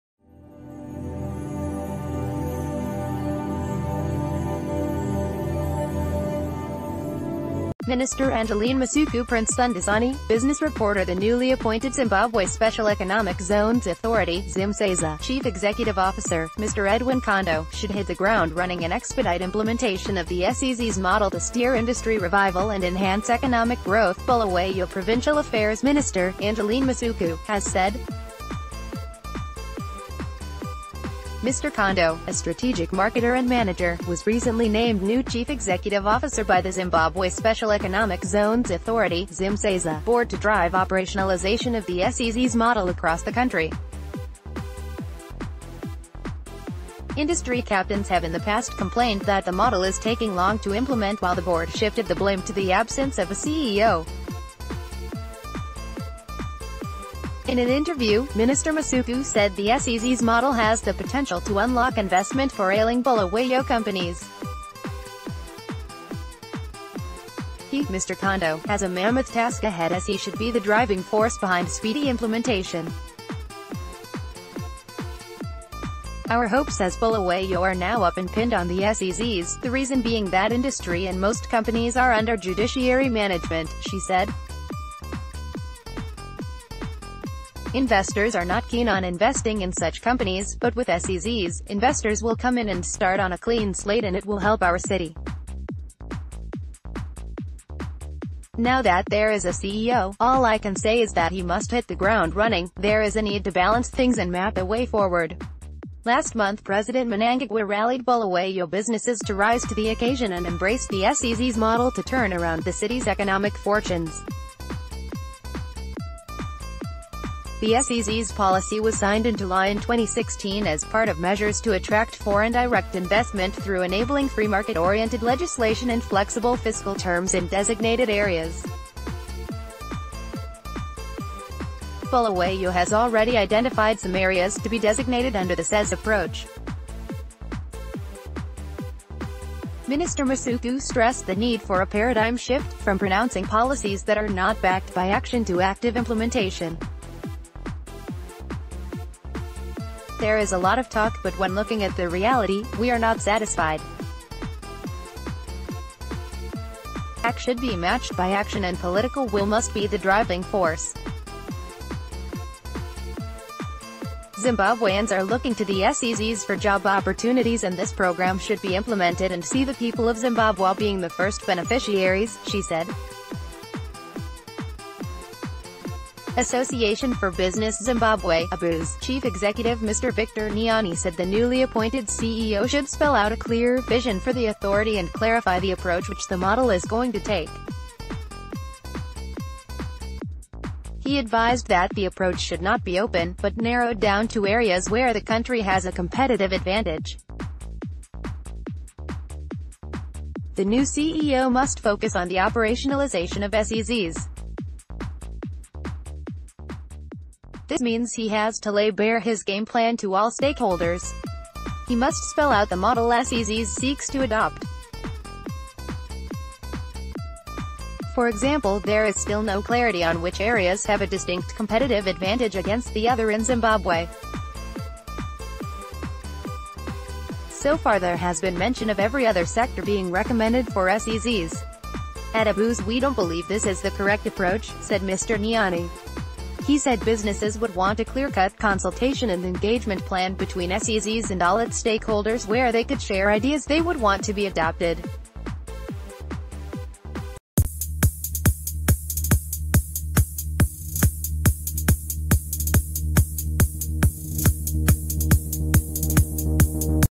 Breaking News | New SEZs boss must hit ground running: Masuku
Minister Angeline Masuku Prince Sunduzani, Business Reporter THE newly appointed Zimbabwe Special Economic Zones Authority (Zimseza) chief executive officer, Mr Edwin Kondo, should hit the ground running and expedite implementation of the SEZs model to steer industry revival and enhance economic growth, Bulawayo Provincial Affairs Minister, Angeline Masuku, has said. Mr Kondo, a strategic marketer and manager, was recently named new chief executive officer by the Zimbabwe Special Economic Zones ...